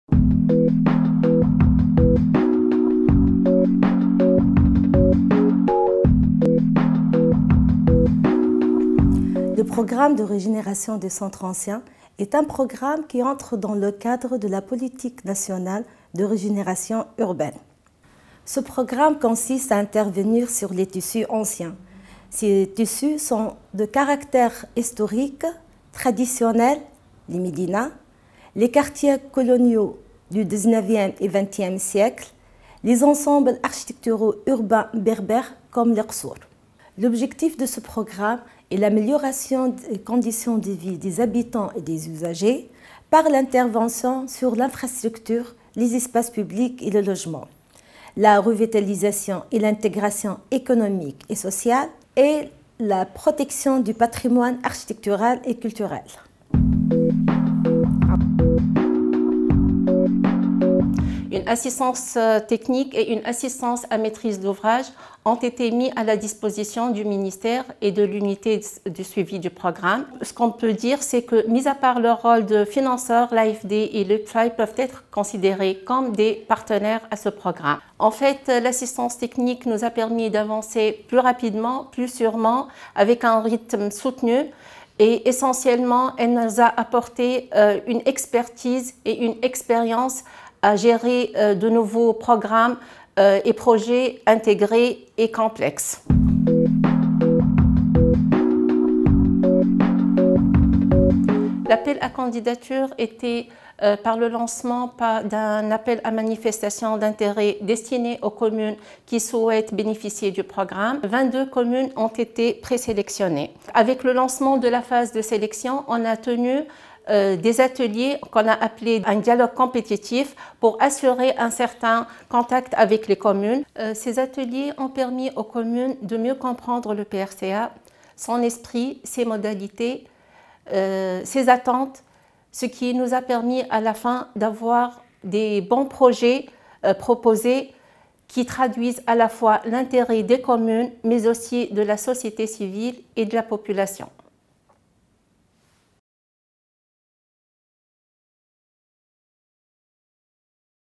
Historic degraded centers: the Tunisian strategy for urban regeneration
The Historic Center Regeneration Programme (PRCA) is part of the Tunisian national policy for urban regeneration. It aims to upgrade the historical and traditional districts, which tend to deteriorate. It is mainly Berber urban architectural districts and colonial districts. This program also aims to improve the living conditions of the inhabitants of these neighborhoods, in terms of housing, public spaces and infrastructure. The French Development Agency and the Urban Project Finance Initiative (UPFI) are partners in this project and have provided technical assistance and project management support. This mobilization has enabled the project to progress at a steady rate and to benefit from specific expertise.

Speakers:
- Inès Zbiba-Ajlani, architect at the Tunisian Ministry of Equipment;
- Souad Jaidi, head of department at the Tunisian Ministry of Equipment.